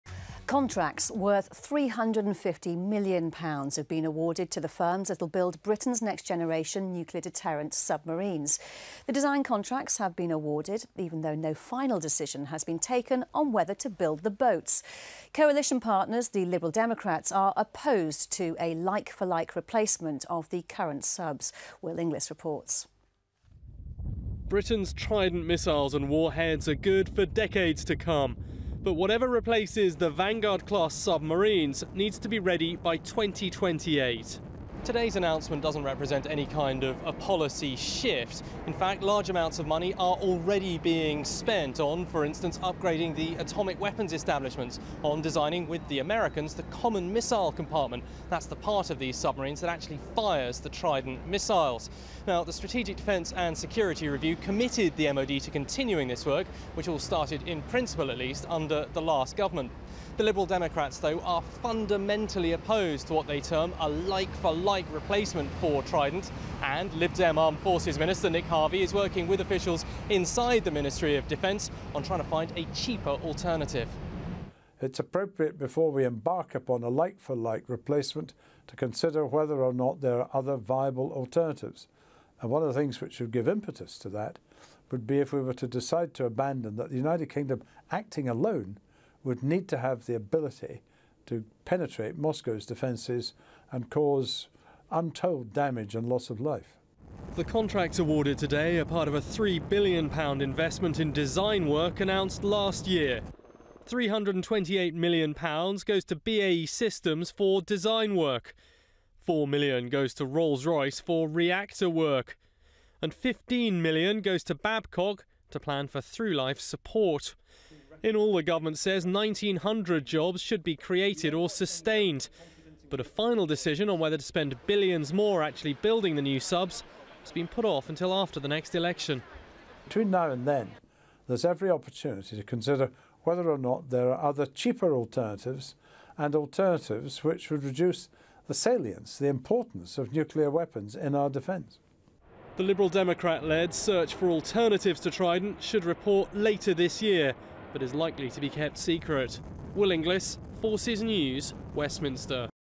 £350m deal new dawn for nuclear subs 22.05.12
The Ministry of Defence has awarded contracts worth £350 million to UK companies to design the next generation of nuclear deterrent submarines, sustaining hundreds of British jobs.

The first Successor submarine is due to be delivered in 2028, replacing the Vanguard Class submarines which currently carry the UK's nuclear deterrent - Trident.

Although a decision on the final design and build will not be made until 2016, detailed work has to take place now to ensure that the Successor submarines will be the most technologically advanced, to protect our national security.

Contracts have been awarded to BAE Systems, Babcock and Rolls-Royce, sustaining or creating 1,900 jobs at sites across the UK. Engineers at the companies will work with the MoD on the design of the new submarines which will use a new nuclear propulsion system.

Defence Secretary Philip Hammond said: "This Government is committed to maintaining a continuous submarine-based nuclear deterrent. The contracts announced today with BAE Systems, Babcock and Rolls-Royce symbolise an important step towards renewing our nation's nuclear deterrent into the 2060s.

"We have a world-class submarine-building industry in this country and this programme will help to sustain or create more than 1,900 jobs across the UK.

"By making the core equipment programme fully funded and affordable, we are able to confirm additional equipment projects which help safeguard our national security."

The largest contract, worth £328M, has been awarded to BAE Systems Maritime -- Submarines, which will work on the overall submarine design. Babcock has been awarded £15M and will focus on designing parts of the in-service support. More than £4M has been paid to Rolls-Royce which will be responsible for the integration of the reactor design.

First Sea Lord, Admiral Sir Mark Stanhope said: "The Royal Navy has been operating continuous at sea deterrent patrols for more than 40 years and the Successor submarines will allow us to do so well into the future with cutting edge equipment."